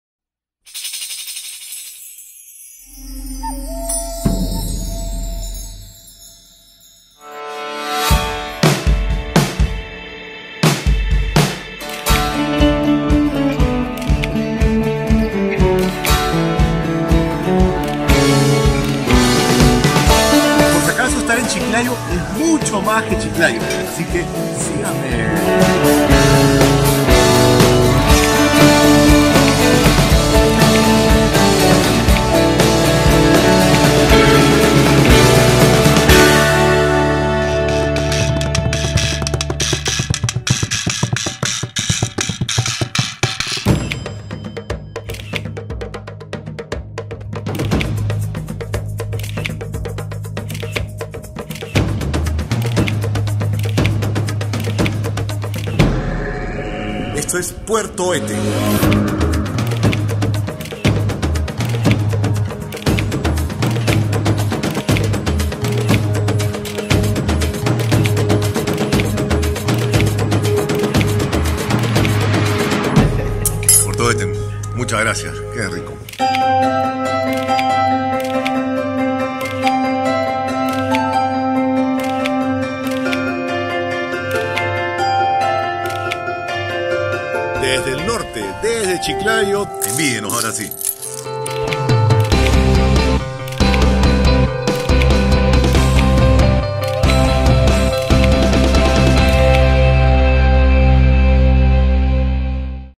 Lumix Por el Perú: Viaje a Chiclayo con Lumix FZ300
¡Recorre el Perú junto a Manolo del Castillo y su Lumix FZ300 de Panasonic!